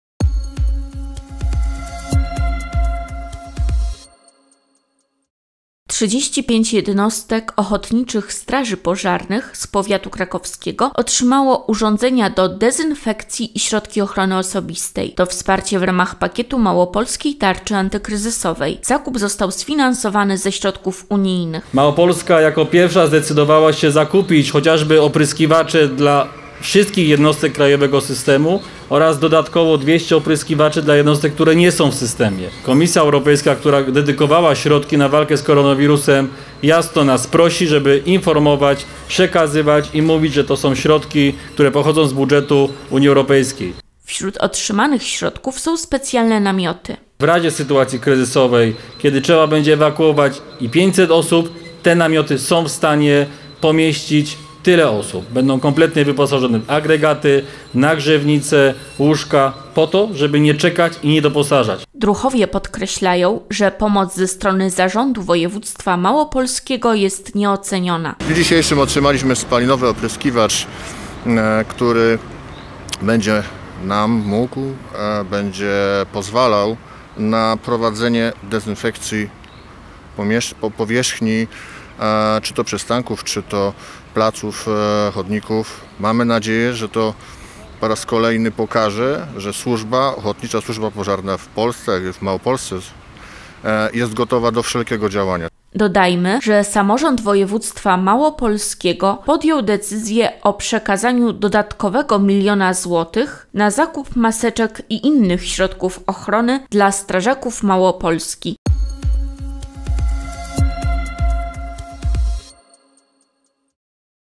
Pomoc dla Ochotniczych Straży Pożarnych
35 jednostek Ochotniczych Straży Pożarnych z powiatu krakowskiego otrzymało  urządzenia do dezynfekcji i środki ochrony osobistej. To wsparcie w ramach pakietu Małopolskiej Tarczy Antykryzysowej. Zakup został sfinansowany ze środków unijnych.